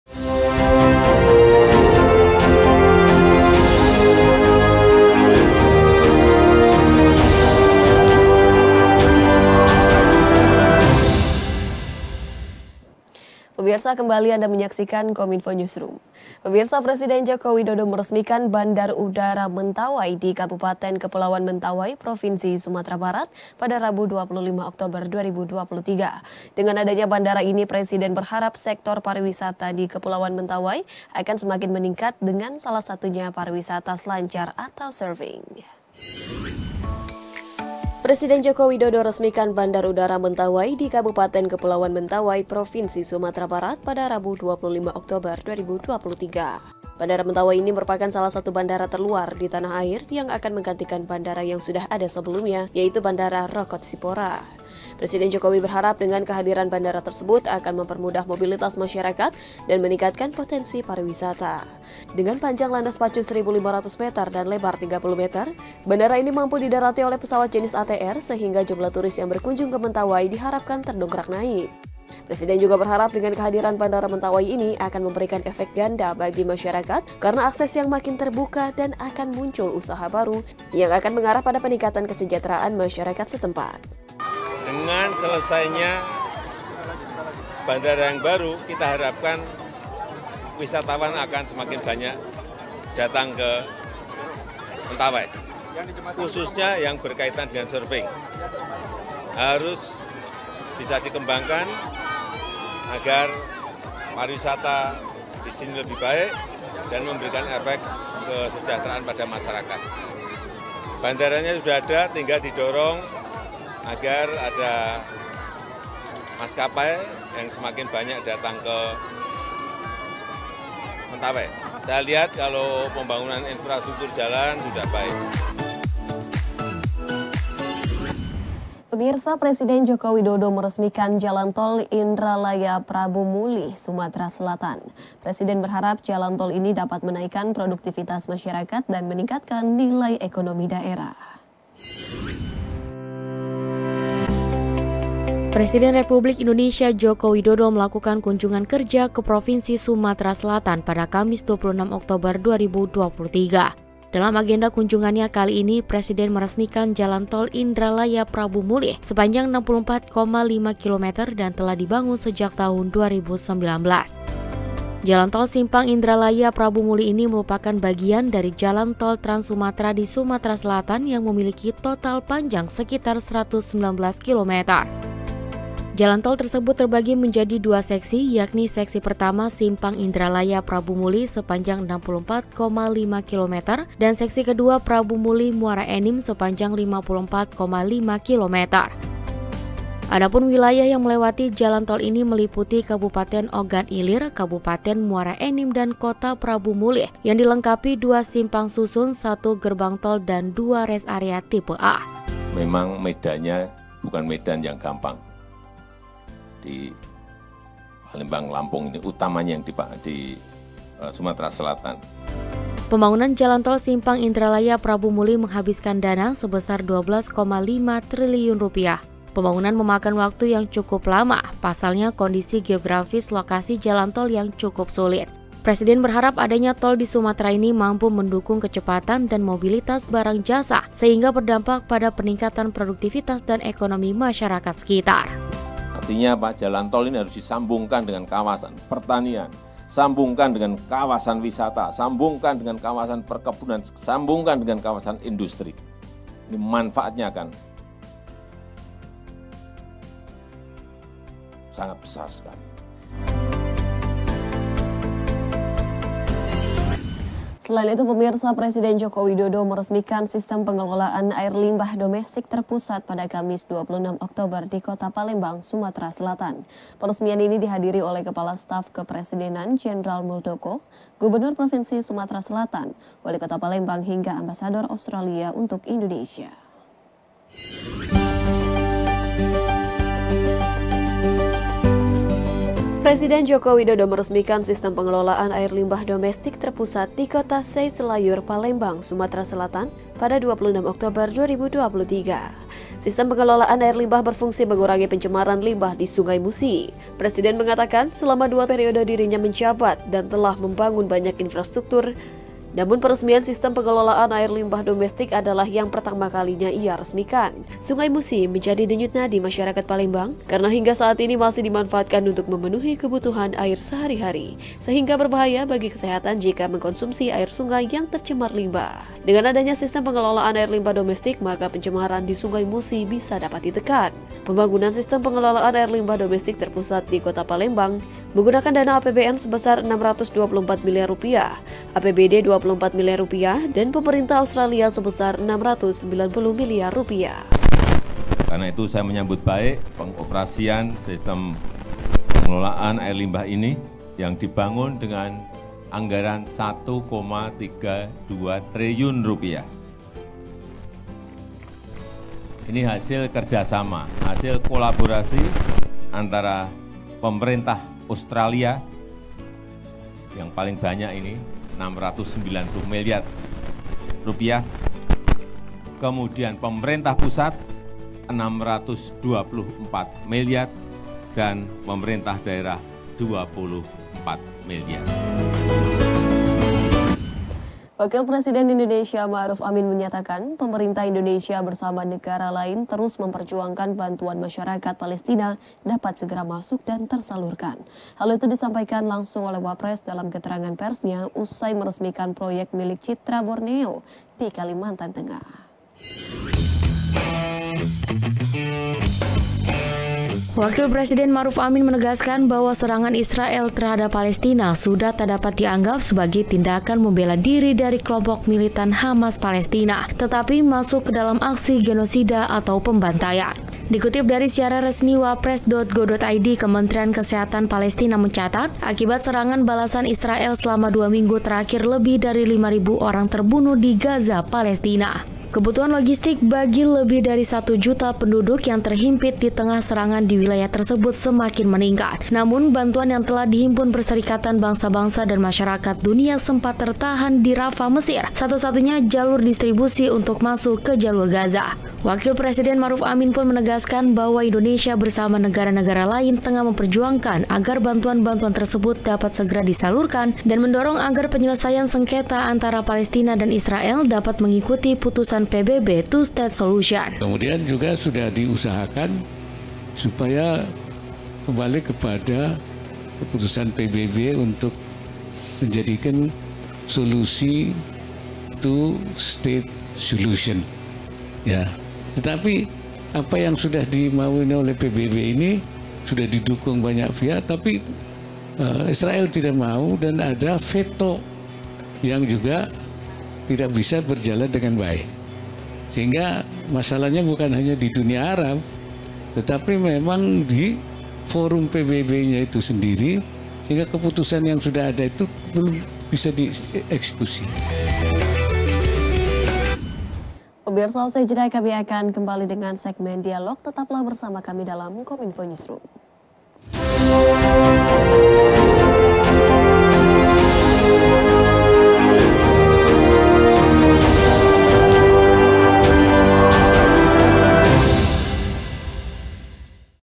Indonesia Kirim Bantuan Kemanusian ke Palestina | Kominfo Newsroom 2/2
Kilas Kominfo Newsroom

1. Bandara Mentawai Diresmikan, Bidik Lebih Banyak Wisatawan

2. Presiden Resmikan Jalan Tol Indralaya Prabumulih

3. Palembang Punya Sistem Pengelolaan Air Limbah Pertama di Indonesia

4. Indonesia Kirim Bantuan Kemanusian ke Palestina

Selamat Menyaksikan!
Salam Sehat!